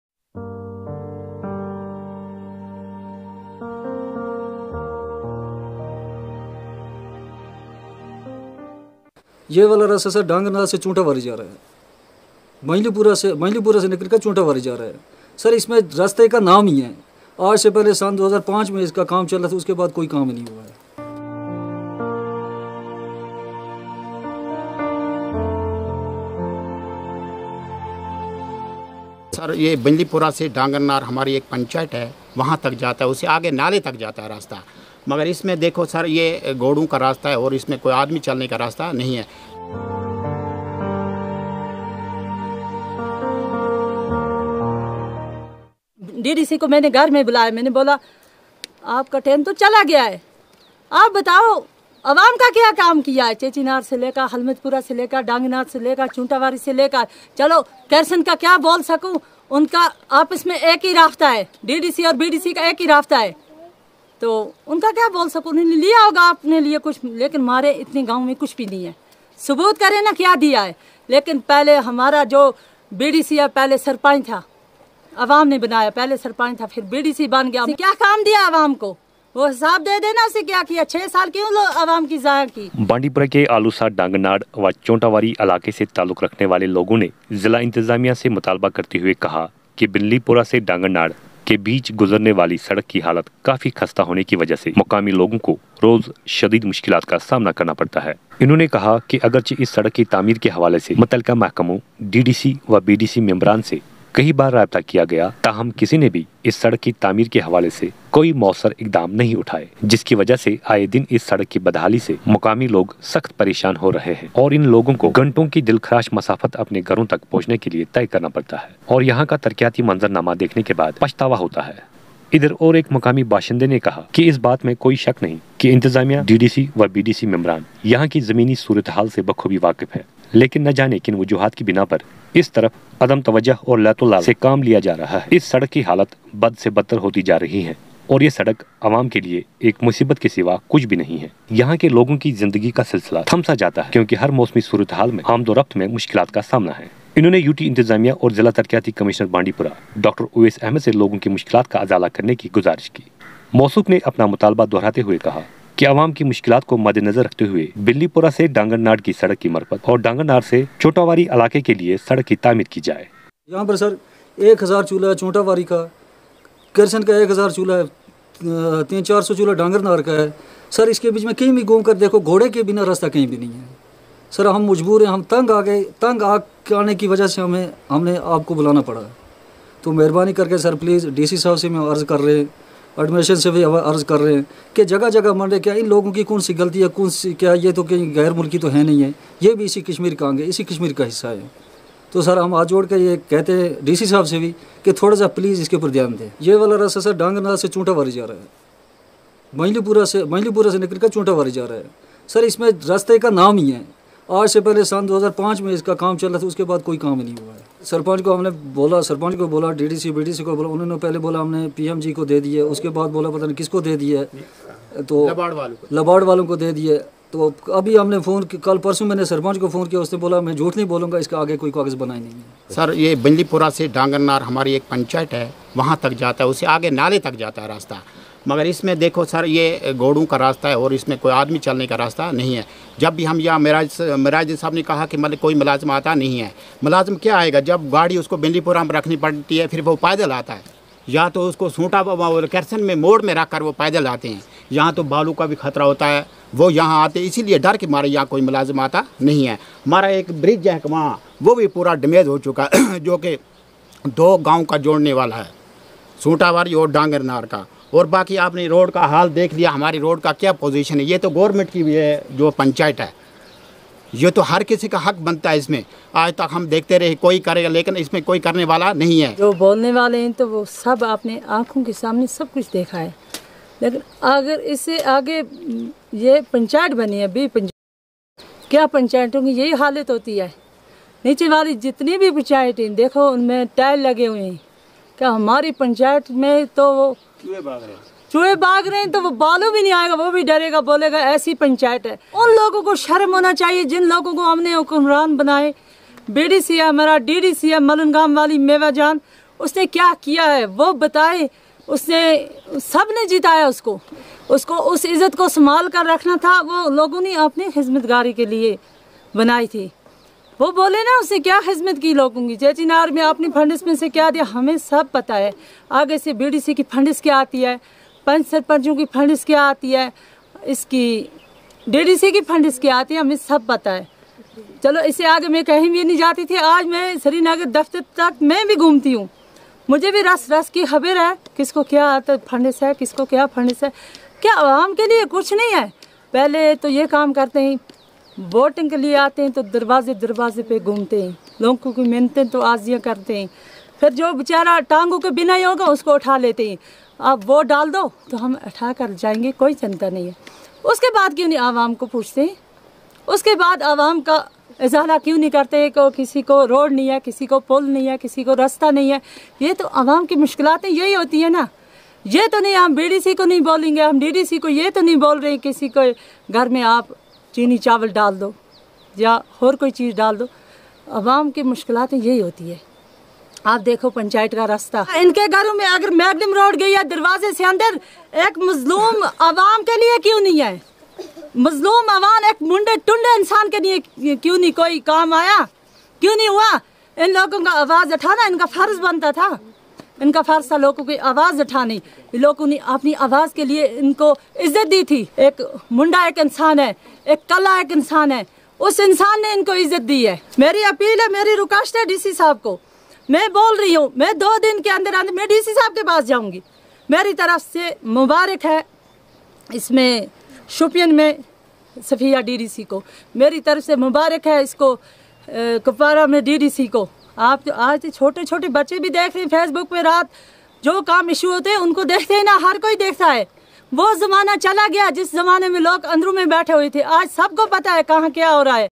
Chontawari and Dangarnard Locals of Aloosa Block demand renovation and Blacktopping of Road
Chontawari villagers demand road connectivity while as Dangarnard Locals of Aloosa Block demand renovation and Blacktopping of Road